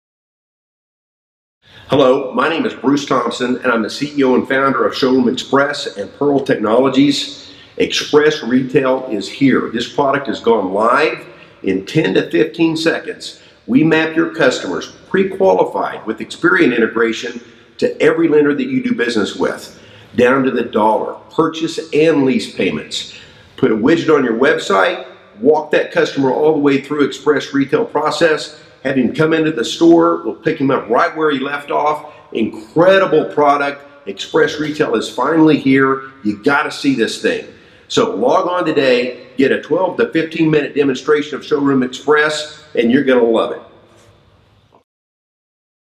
ShowroomXpress Product Overview
Pearl Technologies CEO and Founder Bruce Thompson gives a quick product overview of ShowroomXpress, which is a "clicks-to-bricks" digital retailing solution for auto dealers. Consumers can go online and get pre-qualified in seconds without an SSN# and see personalized payments down to the dollar for any vehicle in inventory.